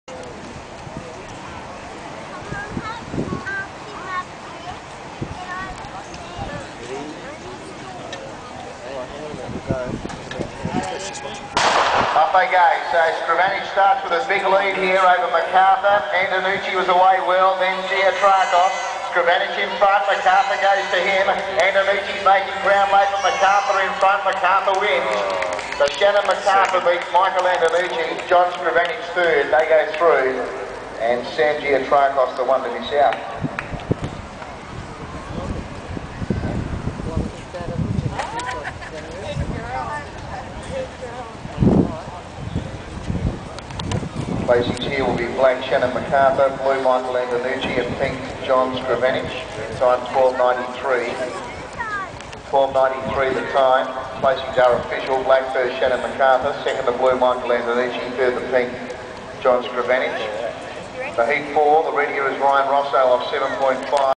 Michael Antenucci - Bay Sheffield 2006
Mike running at the Bay Sheffield 2006 at Glenelg